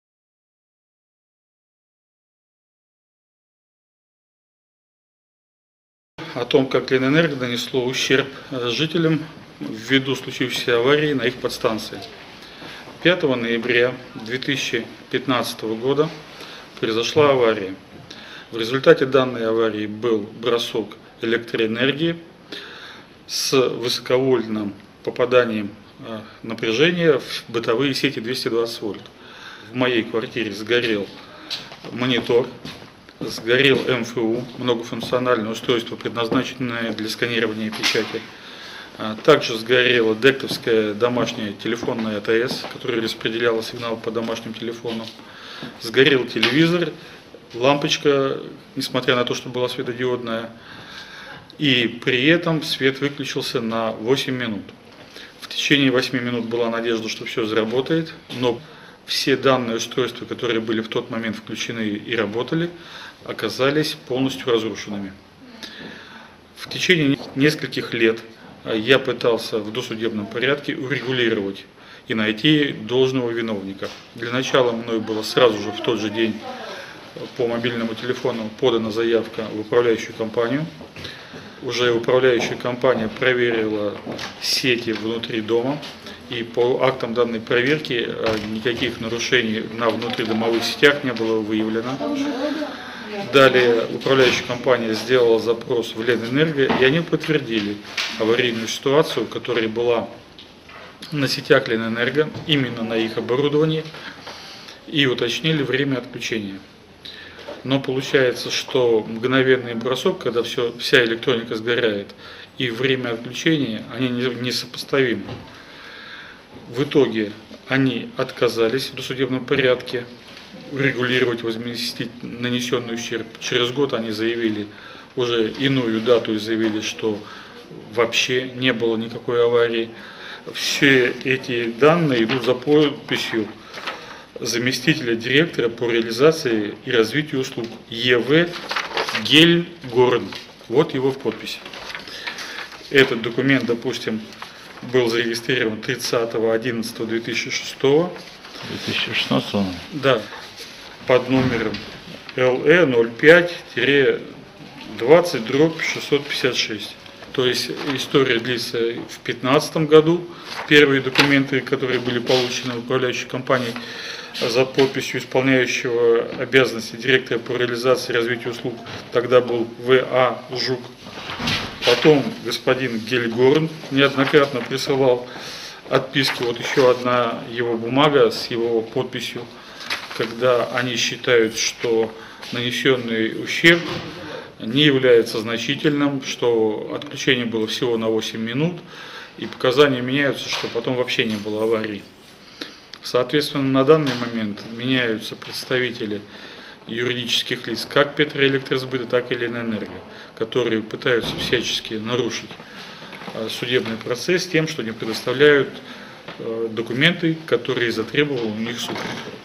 Жители муниципального округа против монополиста ЛЕНЭНЕРГО ( 19 .03.2019 г.)
МО Санкт-Петербурга МО Пулковский меридиан. Гражданское дело рассматривается в Московском районном суде Санкт-Петербурга( ПРОДОЛЖЕНИЕ СЛЕДУЕТ)!!!